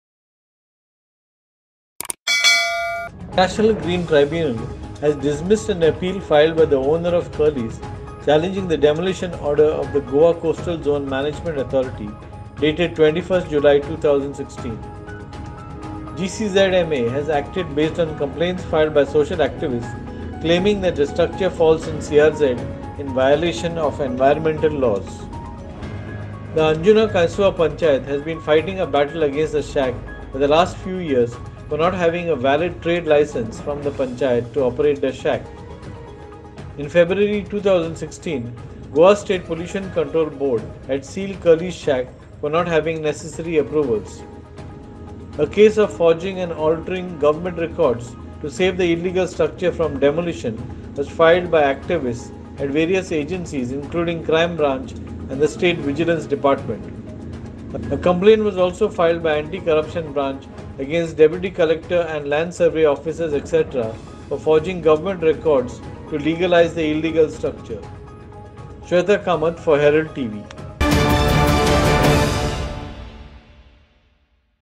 Curlies Night Club at Anjuna, set to be demolished
The Curlies Night Club, which shot into the limelight due to the Scarlett Keeling and Sonali Phogat death cases, be demolished? The National Green Tribunal has dismissed an appeal filed by the owner of Curlies challenging the order of the Goa Coastal Zone Management Authority. The NGT has upheld the order of GCZMA dated 21st July 2016 to demolish the alleged illegal structure in Anjuna and restore the land to its original condition.